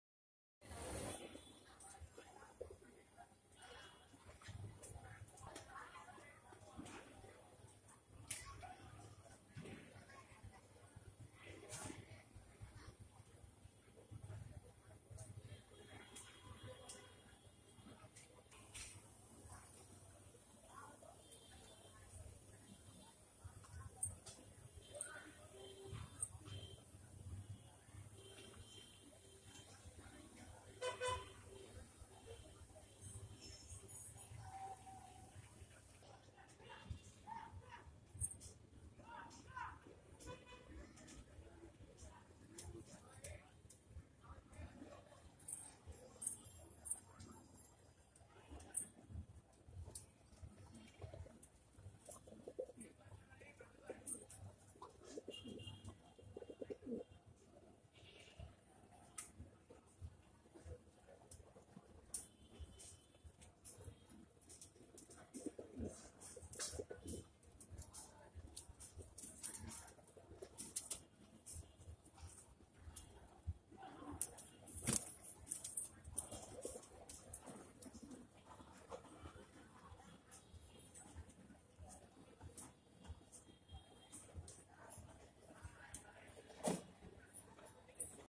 Fantail pigeons|sale|mumbai
Full white Indian Fantail set pair for sale location Mumbai
For details contact ab pigeon loft at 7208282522